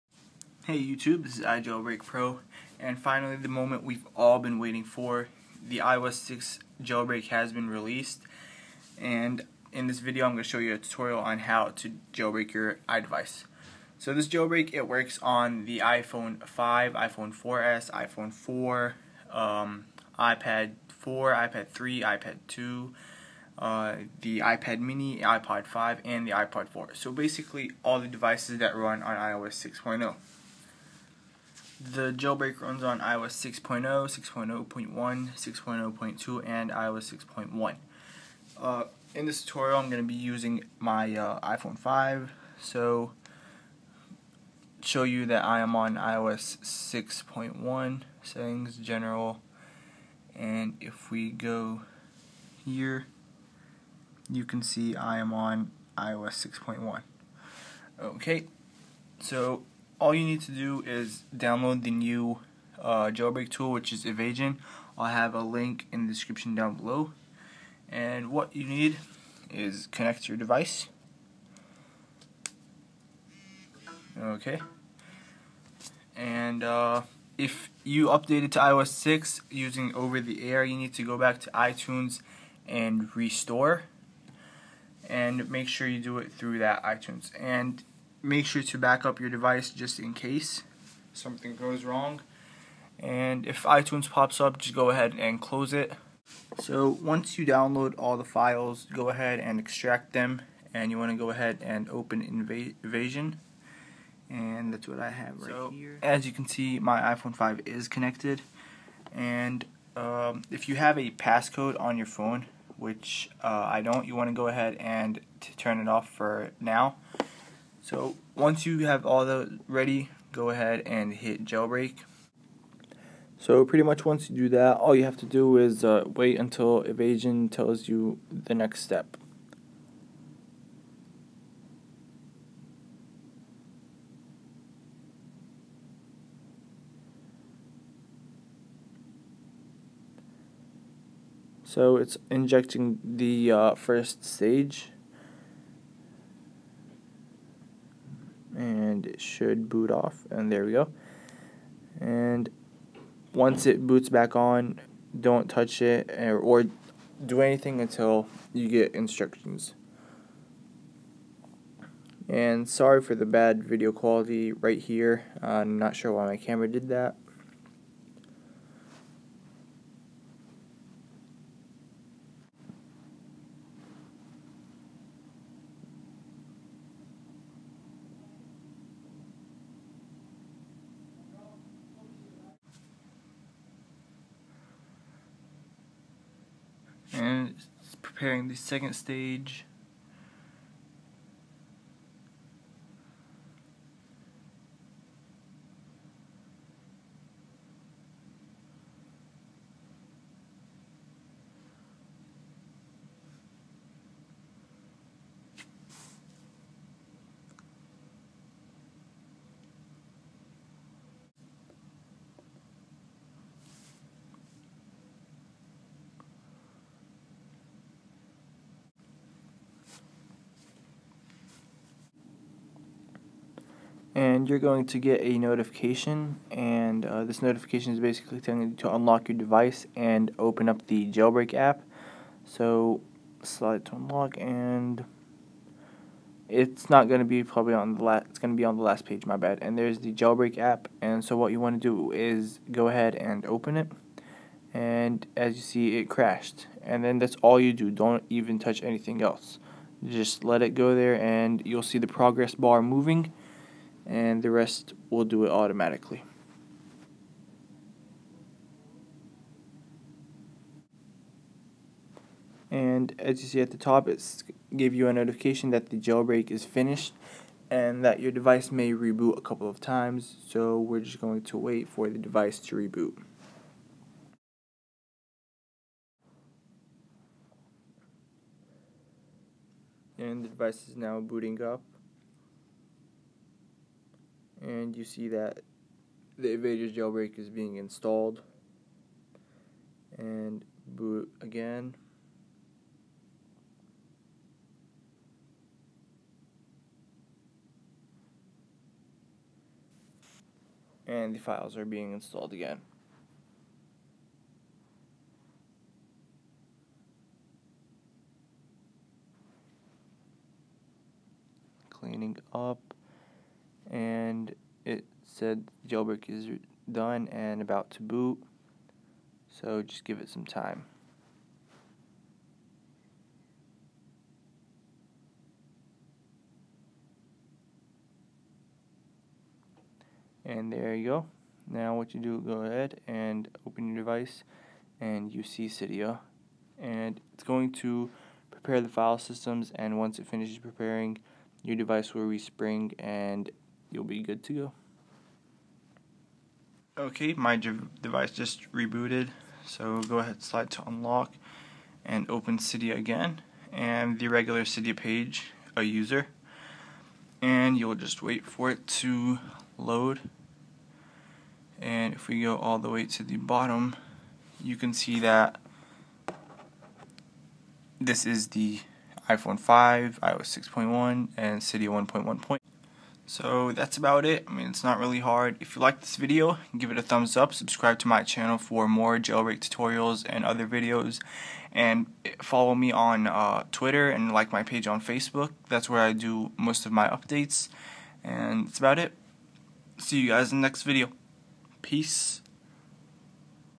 Evasi0n Jailbreak for iOS 6.0-6.1.2 untethered on iPhone 5/4s/4, iPad 4,3,2,mini, & iTouch 5g,4g
How to Jailbreak iOS 6.0/6.0.1/6.0.2/6.1 untethered on iPhone 5,4s,4 iPad 4,3,2, mini & iPod 4g,5g using Evasi0n.